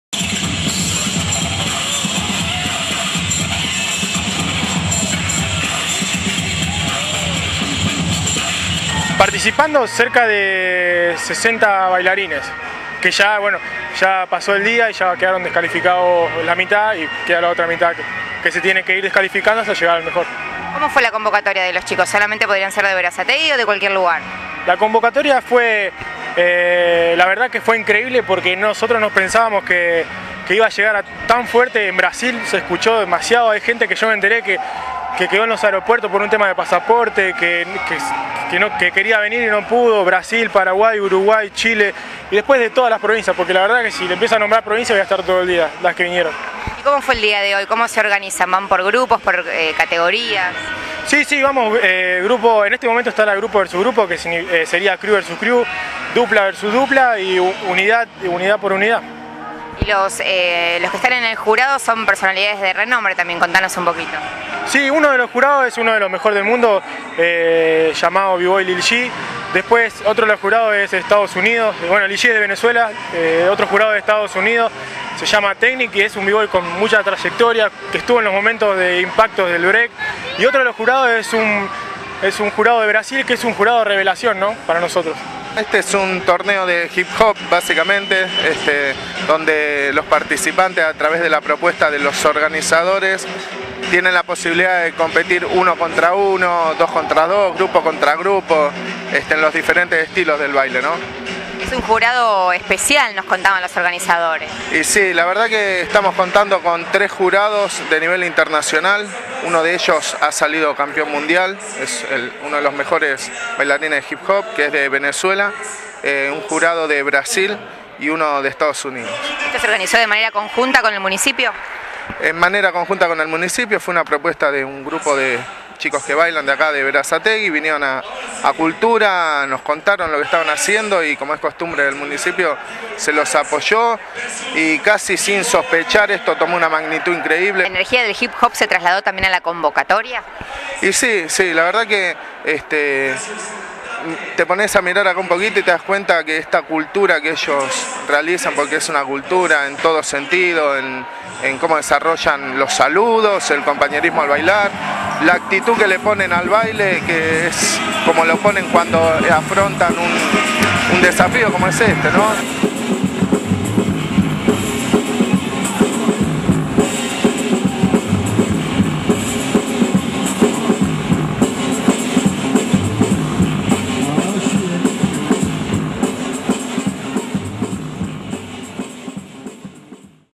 Espectacular Torneo de Hip Hop
Cerca de sesenta bailarines participaron el Torneo de Hip Hop que se desarrolló en el Club Ducilo de Berazategui. El espectáculo fue de un nivel superlativo. El jurado estuvo compuesto por expertos de Estados Unidos, Brasil y Venezuela.